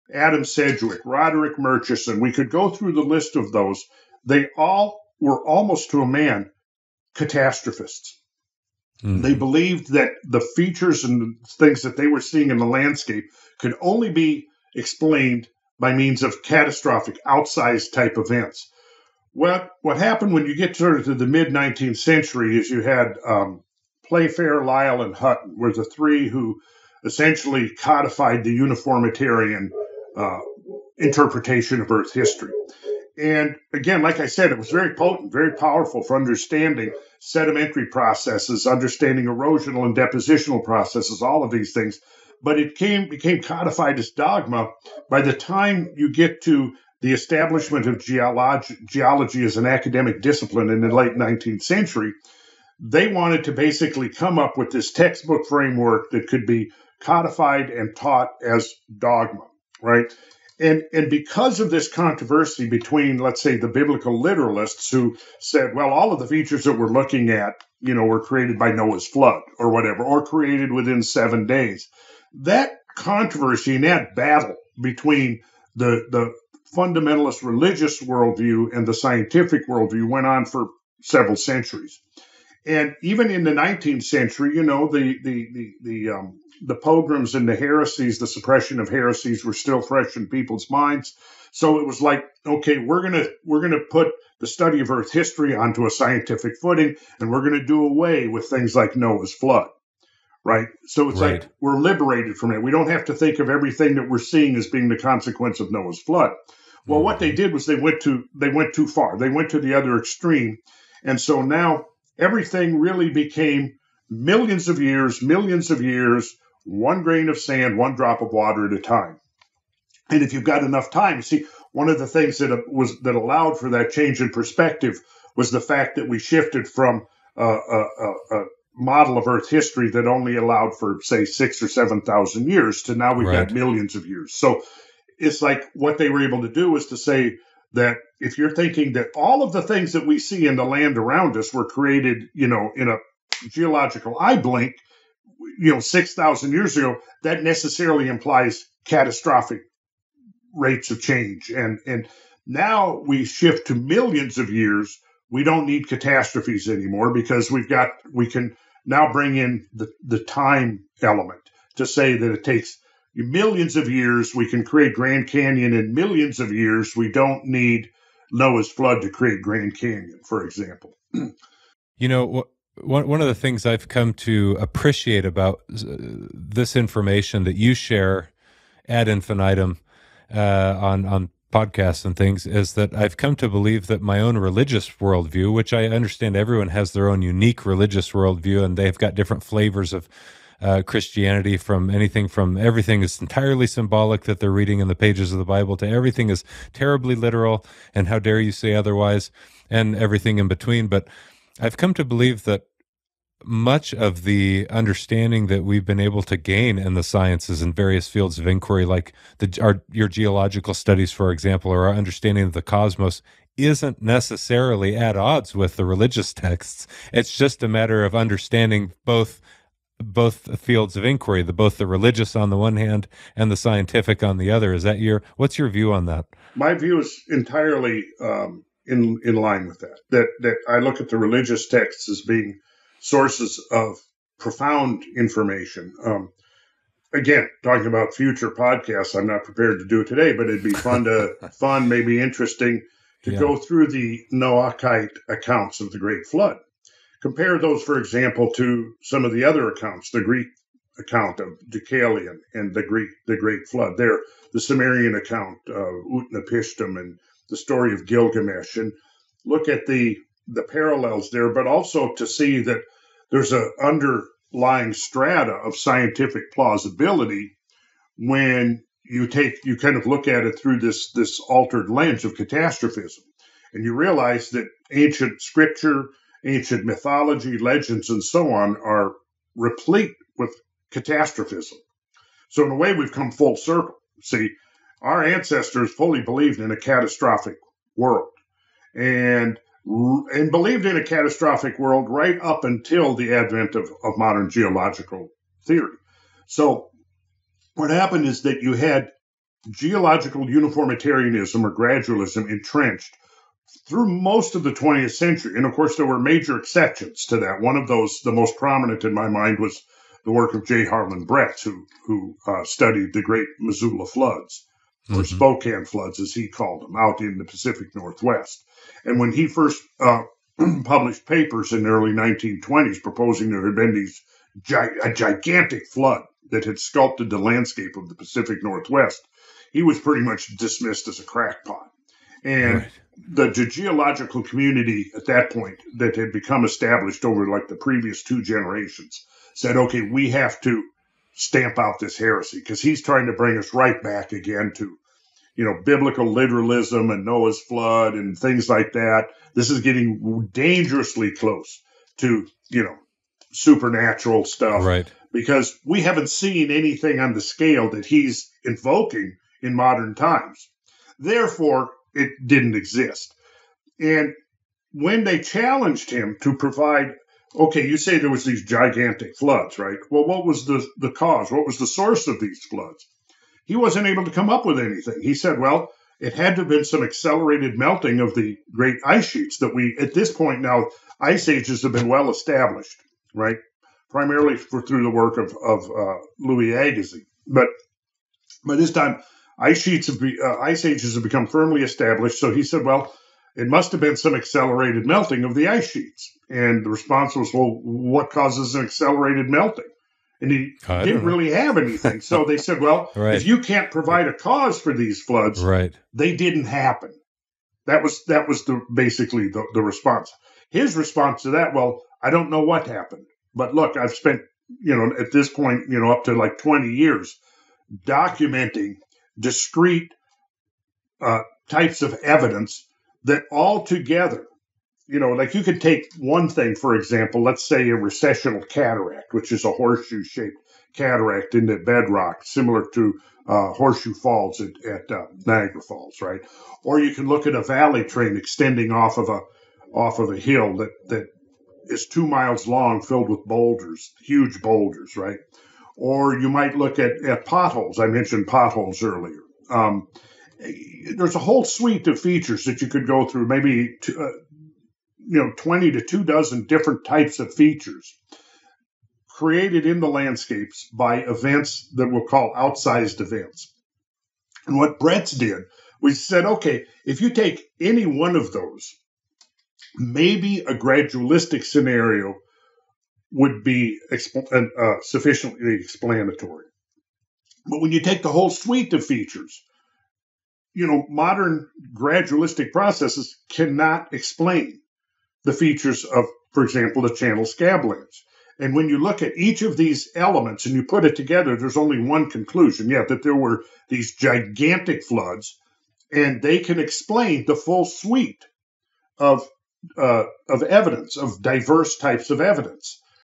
Randall Carlson on Noah’s Ark and The Problem With Modern Geology
The most significant founders of geology believed that many of Earth's features could only be explained through catastrophic events.

However, by the time geology was established as an academic discipline in the late 19th century, the uniformitarian interpretation of Earth's history became the most accepted theory in geology.

The problem with this view is that it has become dogmatic, discarding considerable evidence showing that some of Earth's features necessarily imply catastrophic rates of change.

Randall Carlson believes this dogmatism also shines some light on the centuries-long controversy between the fundamentalist religious and scientific worldviews.

Is there a way to reconcile these views?

Additional Resources ✅

If you’ve enjoyed this episode, please share and follow it on your favorite platform.